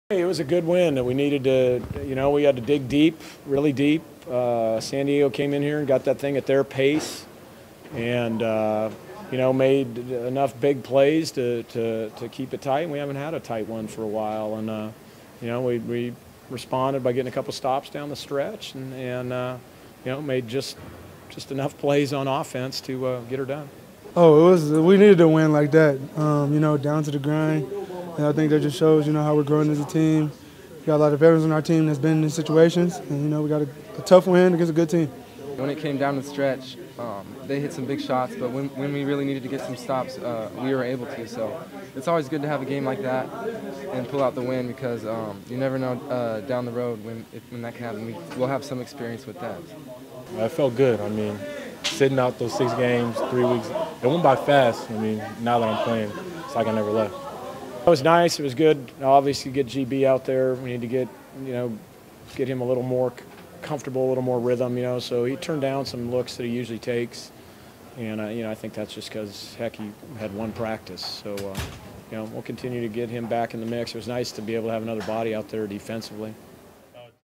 (Interview) Gonzaga Basketball vs San Diego
Interviews with Kyle Dranginis, Gary Bell Jr., Sam Dower, Jr., and Head Coach Mark Few